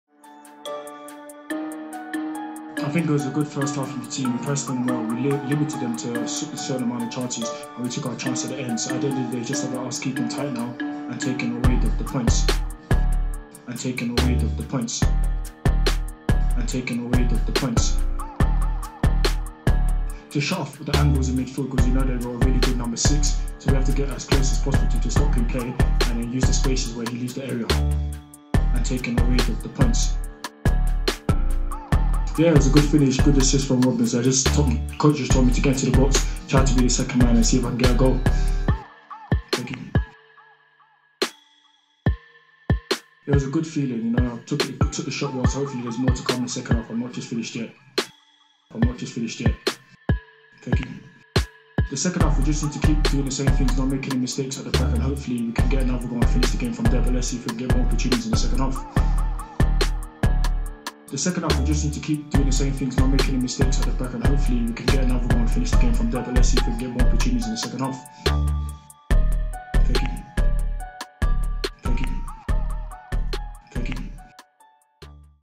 Moses Makasi half time interview - grime remix
IK Brage's scoring british midfielder with the lovely accent, interview at half time at Hammarby away (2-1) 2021.